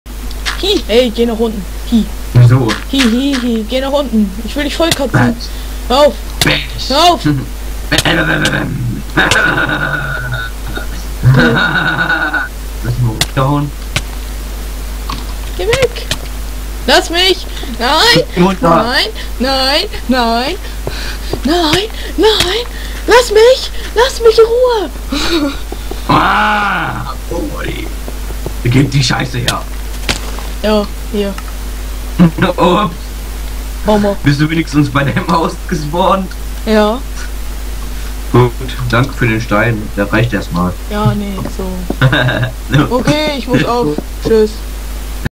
Lets Kill mit BlackBird :D
Lets Run and Kill mit Blackbird auf dem kings craft Server :D